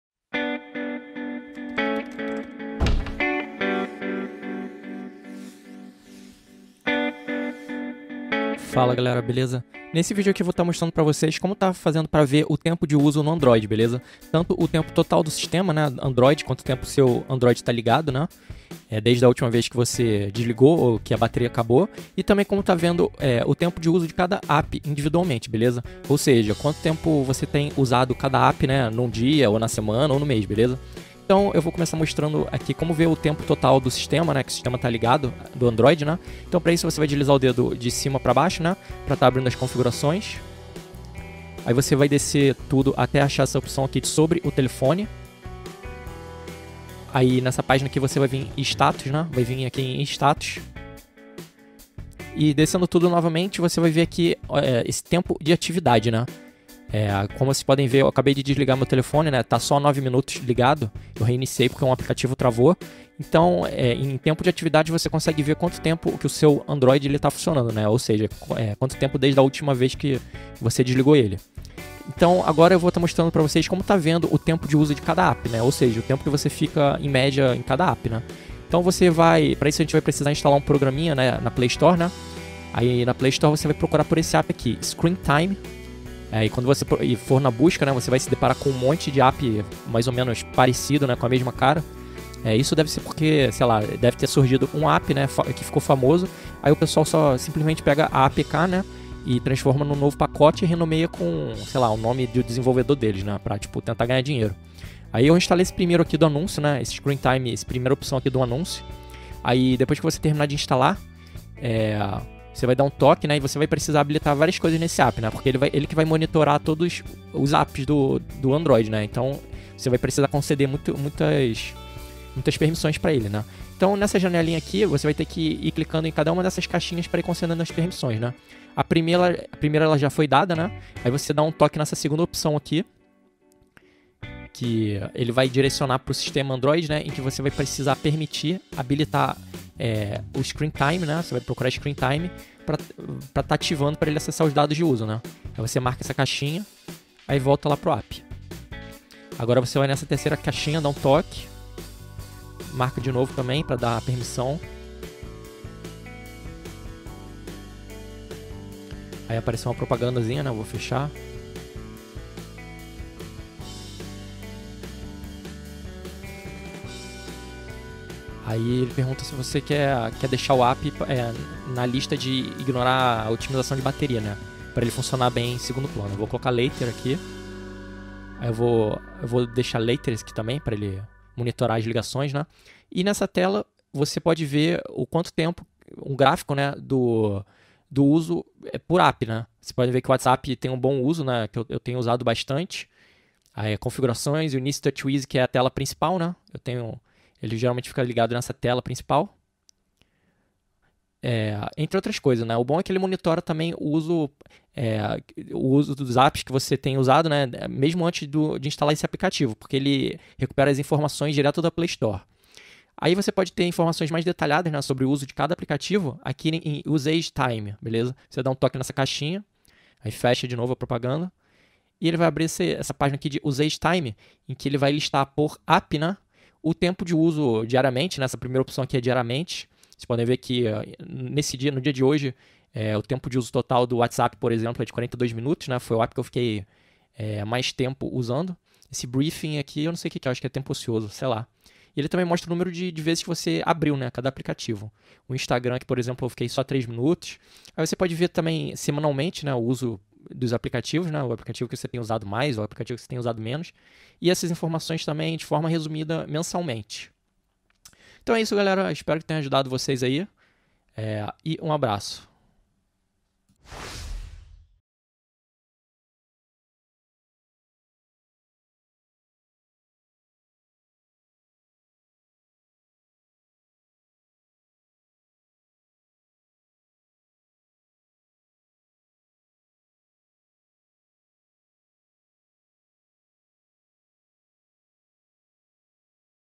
Como ver o Tempo de uso no Android
Veja como estar vendo o tempo de uso do sistema android e quanto tempo você passa em cada app